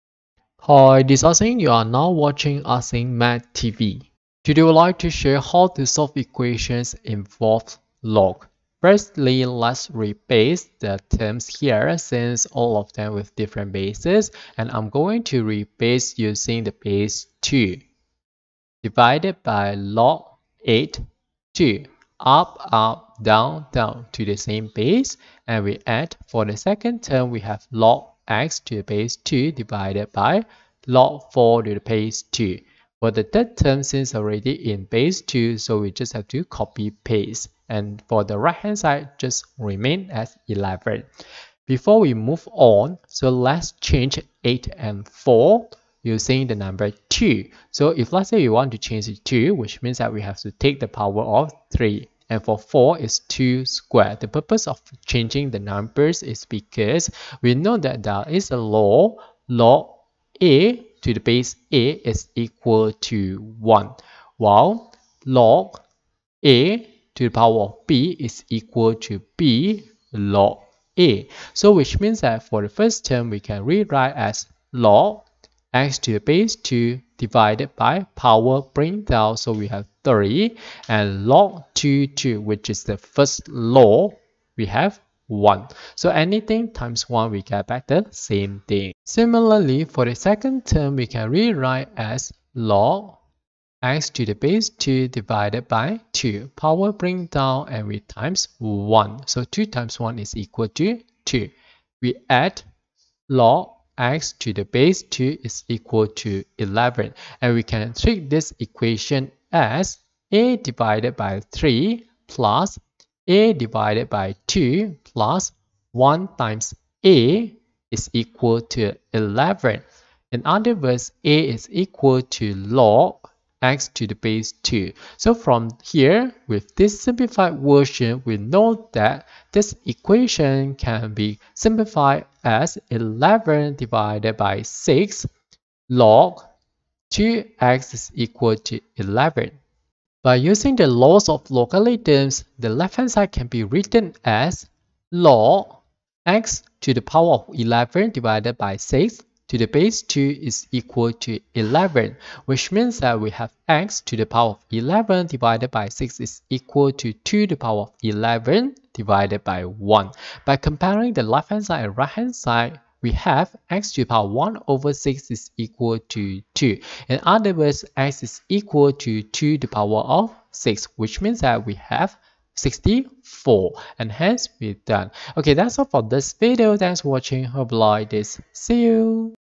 Solve log8x+log4x+log2x=11
Solve the logarithmic equation log8x+log4x+log2x=11 by using laws of logarithm and laws of indices. Tricks and hacks!

Could not solve an equation?